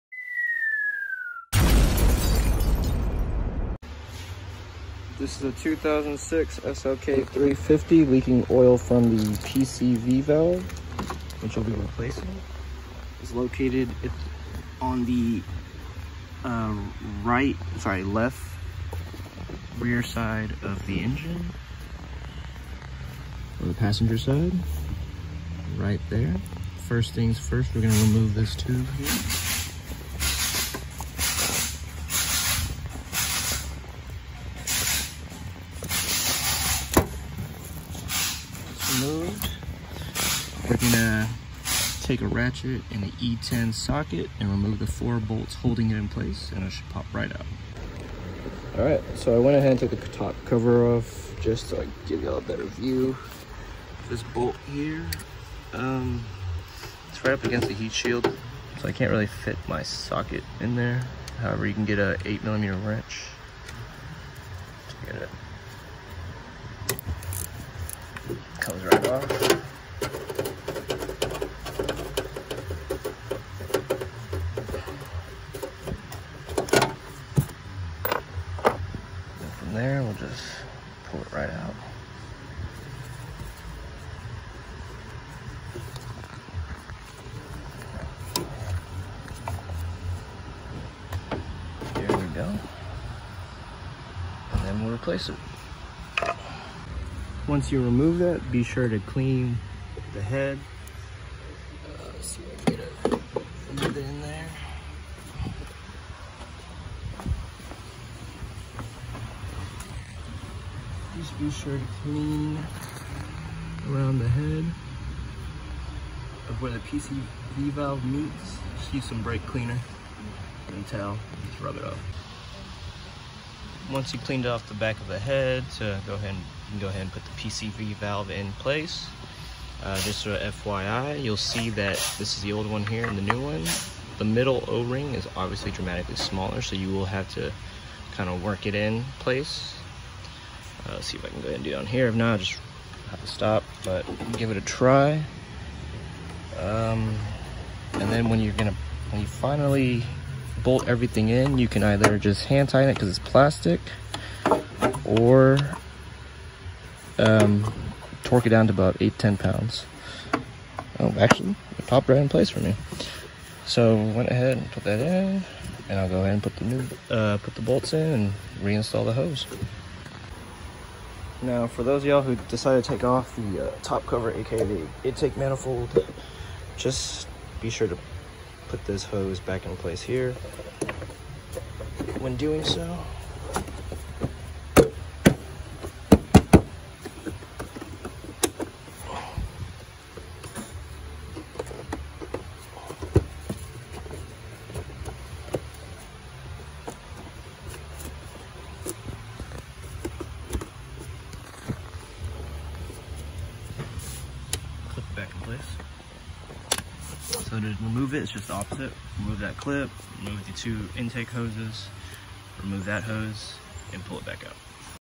2006 Mercedes-Benz SLK350 PCV Valve Replacement
How to locate and replace the PCV valve on a 2006 Mercedes Benz SLK 350.